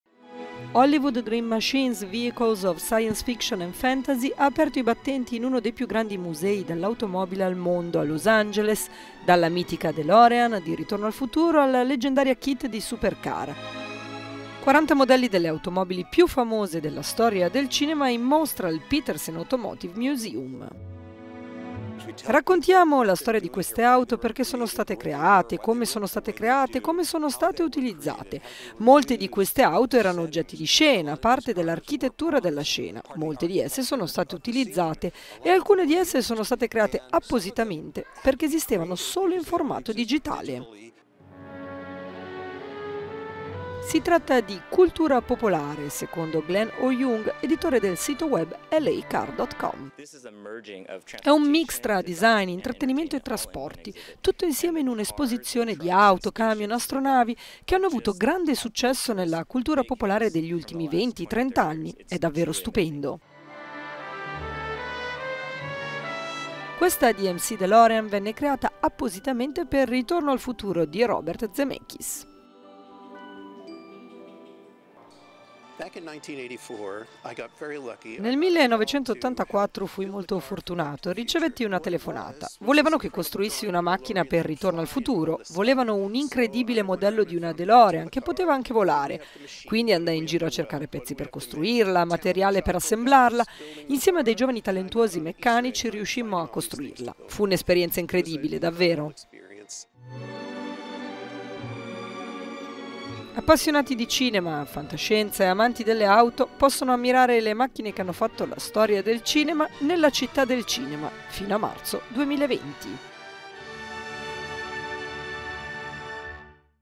Le auto dei sogni in mostra a Los Angeles
Dalla DeLorean di "Ritorno al futuro", a Kitt di "Supercar", passando per la Jeep di "Jurassic Park". Le auto più famose della storia del cinema sono in mostra al Petersen Automotive Museum di Los Angeles…